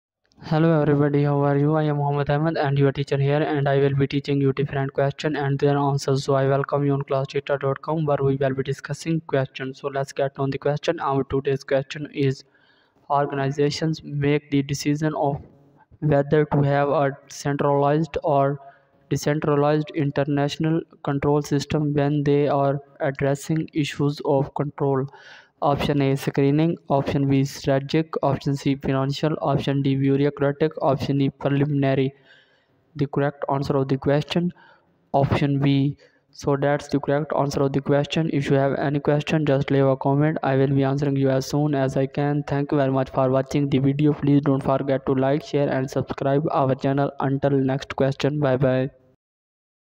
Organizations make the decision of whether to have a centralized or decentralized international
76. Organizations make the decision of whether to have a centralized or decentralized international control system when they are addressing issues of ____ control. a. screening b. strategic c. financial d. bureaucratic e. preliminary